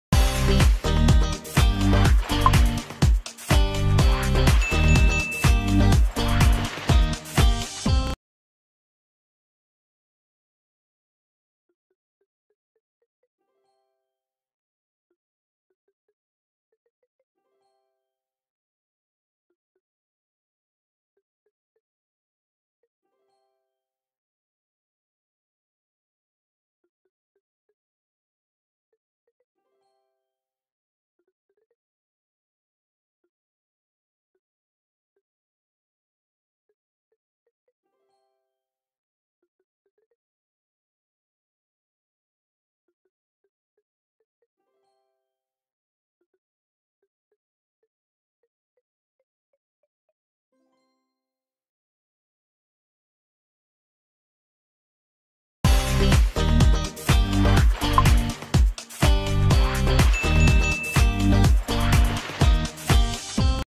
Wordbrain Tiger Level 4 Answers - Wordbrain Tiger Updated 2019
Wordbrain Tiger Level 4 Answers, The puzzle was updated on January 31, 2019. The answers and the complete walkthrough are as follows.